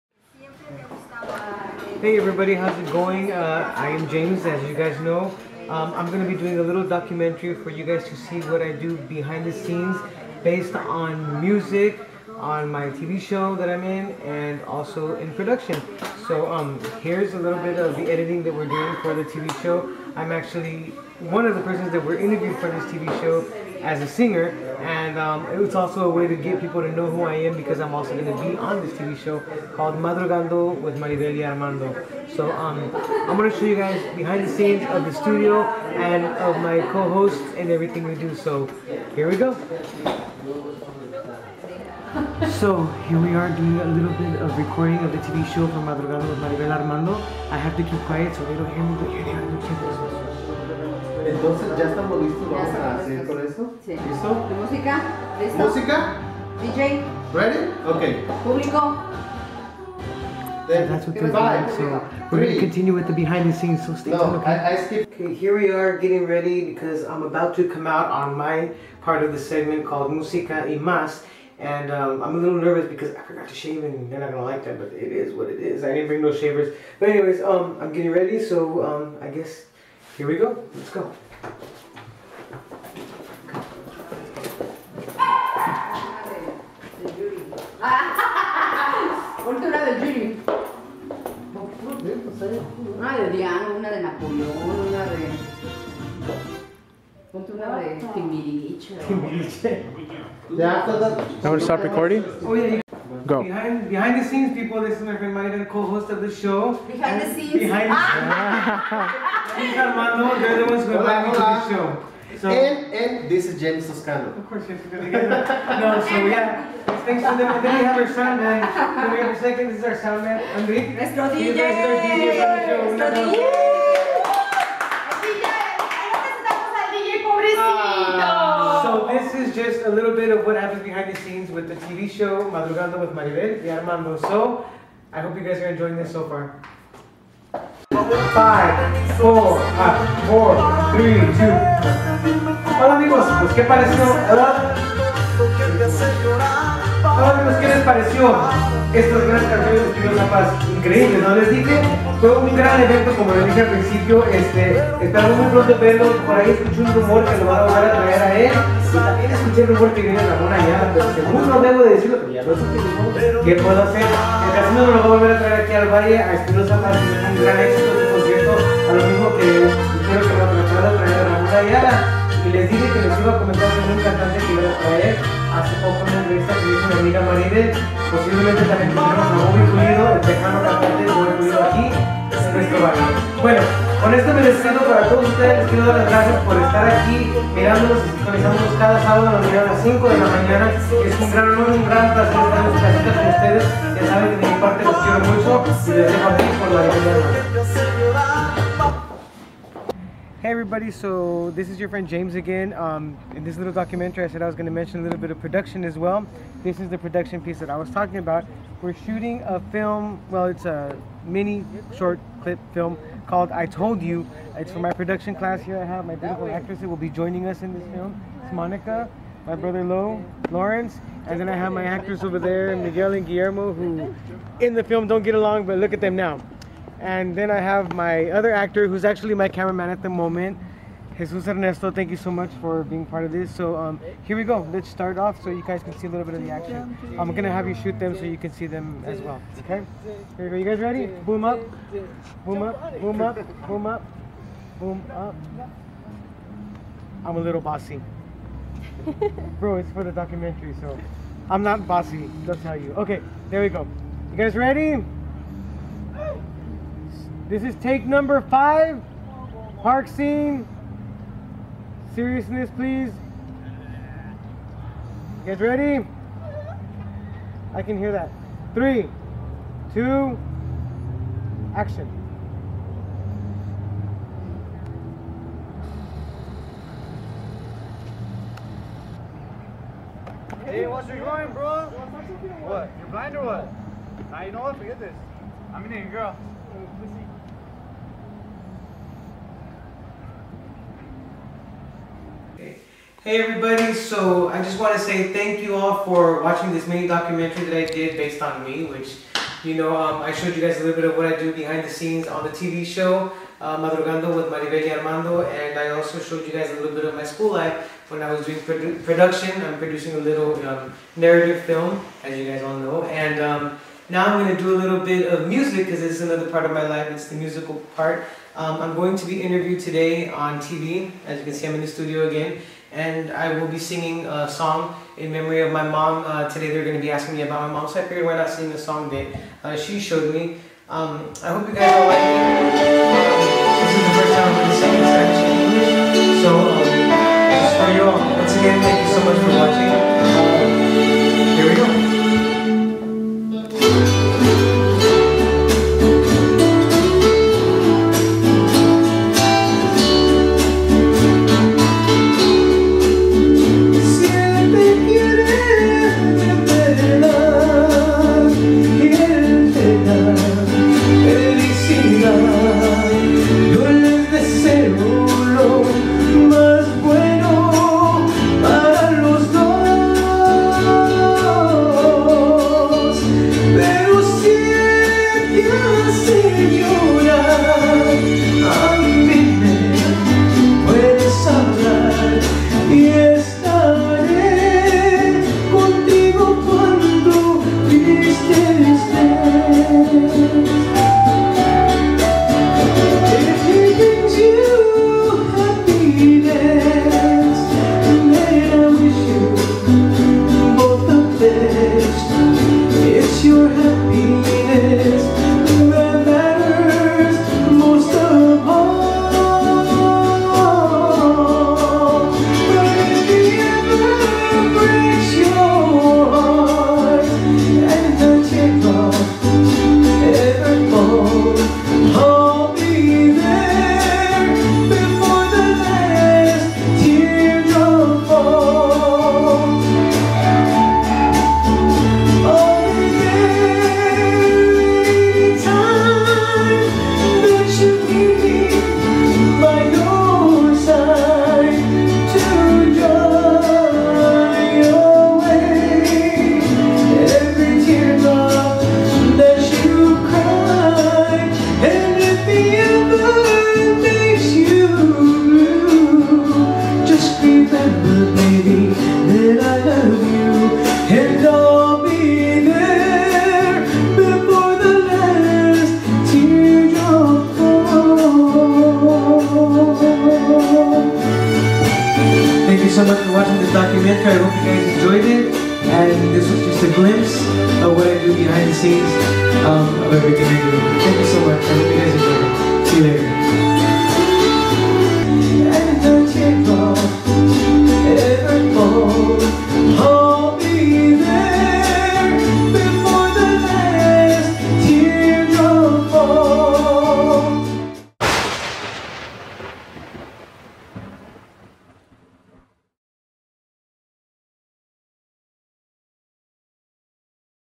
James Toscano MINI DOCUMENTRY
Here is A small Documentary of the different projects i do behind the scenes, in 2013 including Music, Film and Tv Producition And the behind the scenes of being on the TV Show Madrugando con Maribel y Armando Thank you For watching I Hope you all enjoyed It